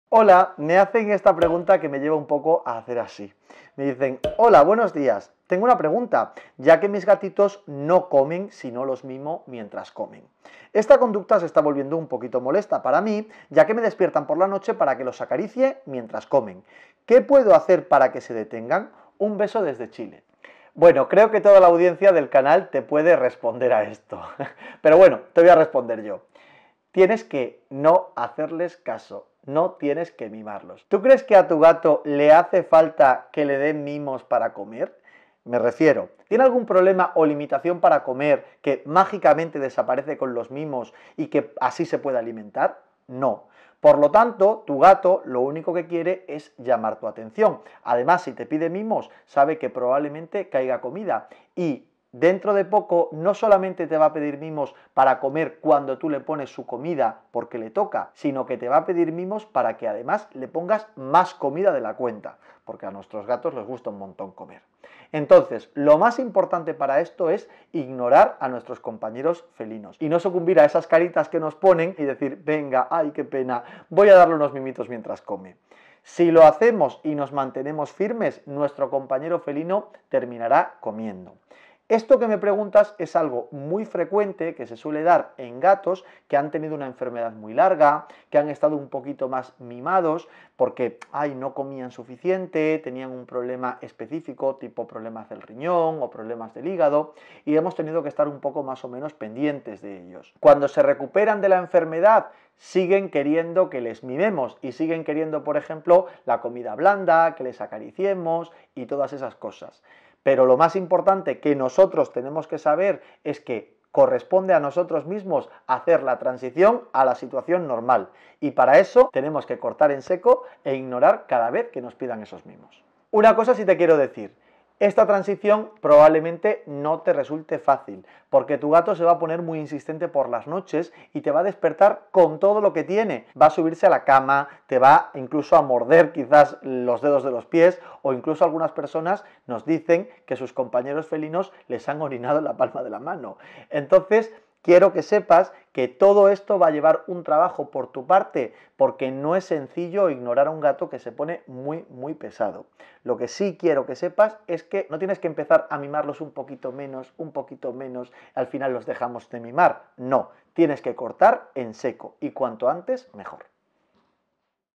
Mi gato solo come si lo acaricio, ¿cómo puedo dejar de hacerlo?
En Instagram nos cuentan la historia de un gato que solo come si le están acariciando, pero su familia humana empieza a sentirse incómoda con la situación. ¿Cómo puede dejar de hacerlo?

Mi respuesta en este vídeo.